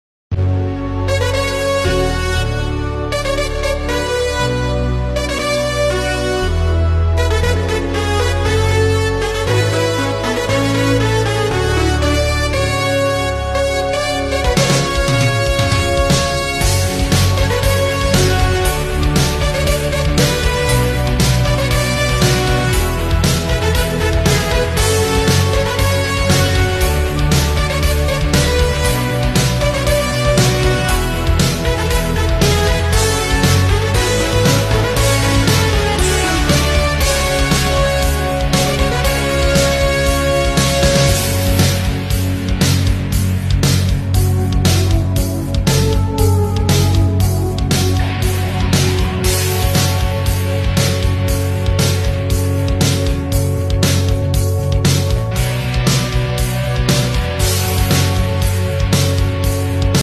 Nbt evo on bench coding programming
Bmw F series car, Optional Rear axle system, HSR repair and calibration test.